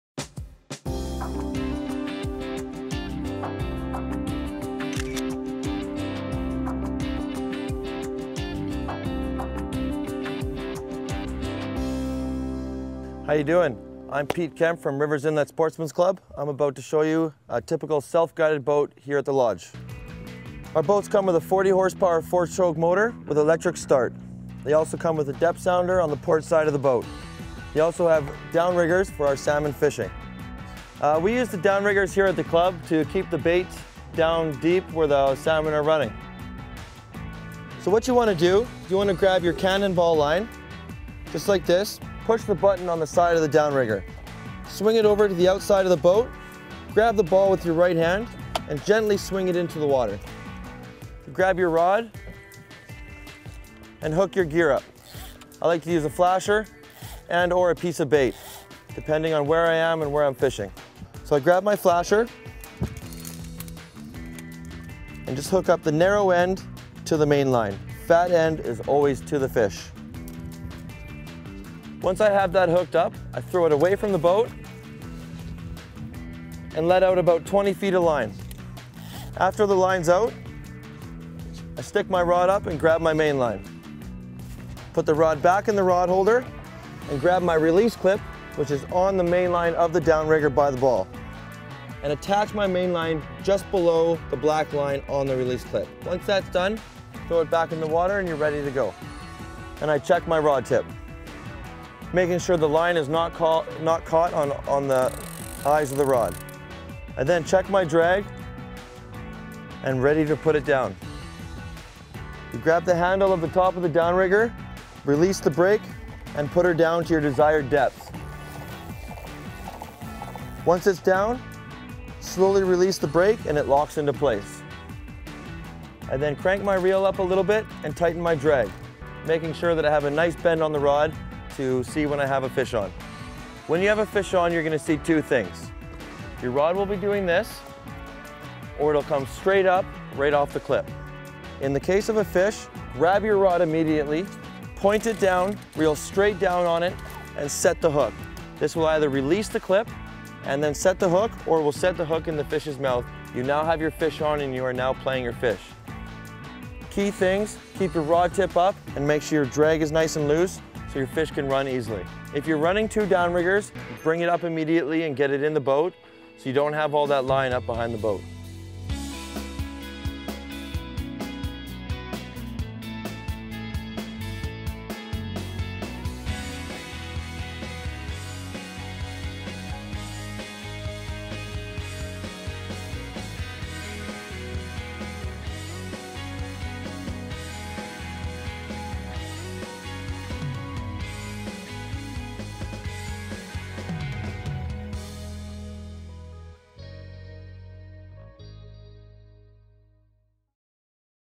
Rivers Inlet Sportsman's Club - Downriggers.mov
Instructional video on the use of downriggers by Rivers Inlet Sportsma's Club. We are a fly in sport fishing lodge on the West coast of British Columbia Canada.